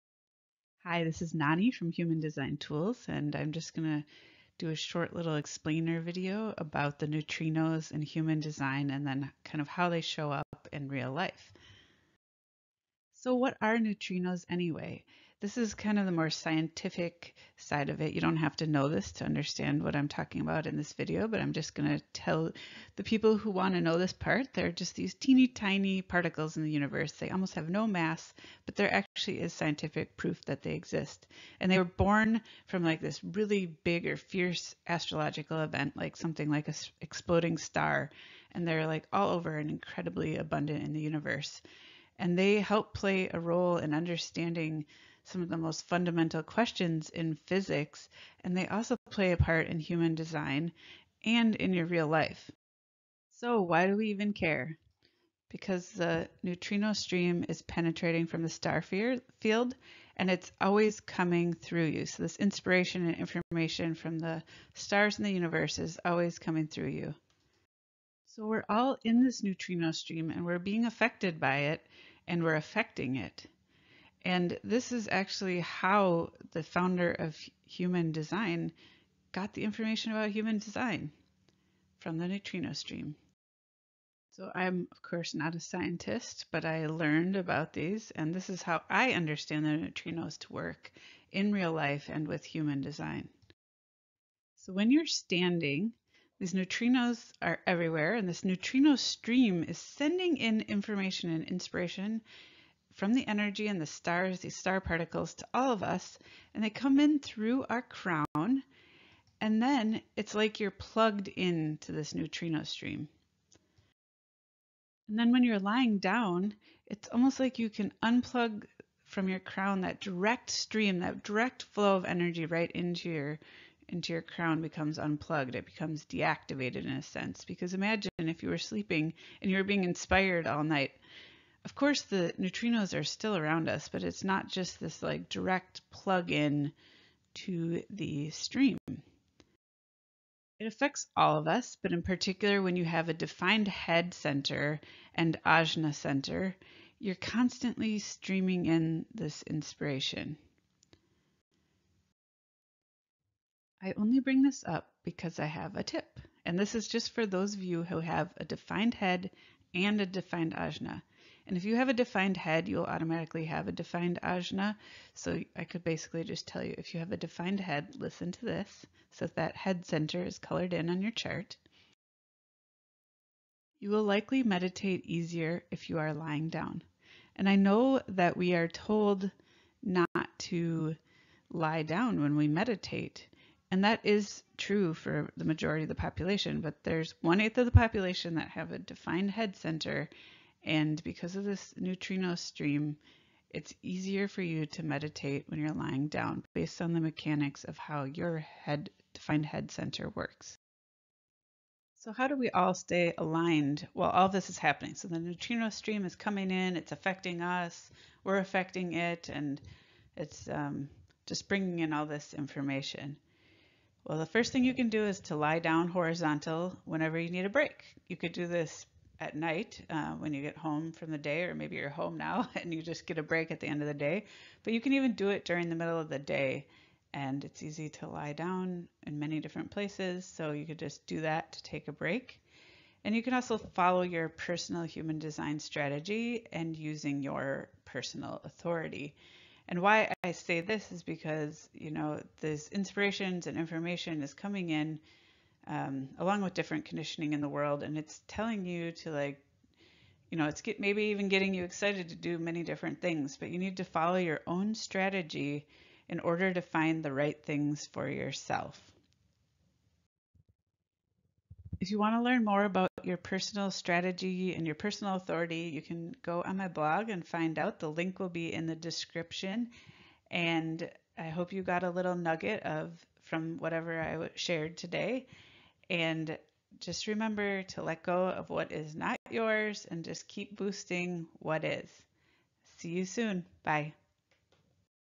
Neutrino Stream and Human Design In Real Life
If you are into Human Design, you likely have heard of the neutrino stream.  You may even already know what it is scientifically or at a basic level, but this video is about how it might play out in real life.

As promised in the video, here's the link to my blog about Human Design: